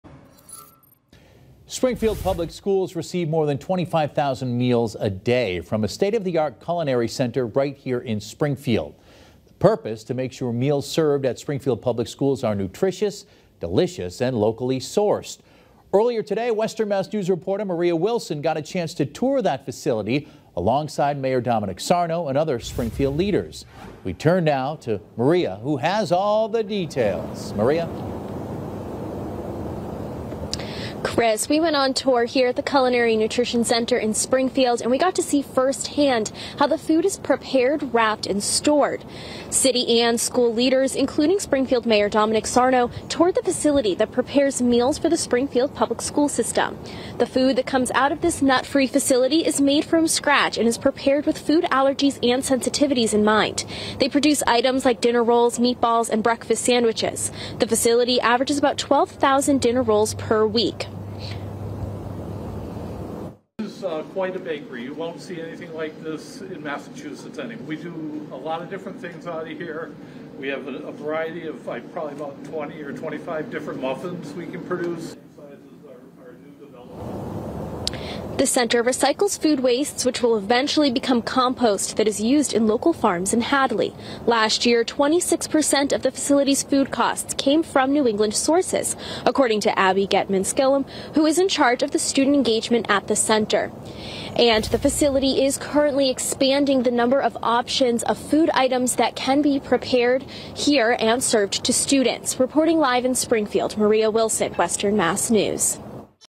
Springfield leaders tour Springfield Public Schools meal facility
The facility is currently in the process of expanding the options of foods that can be prepared and served.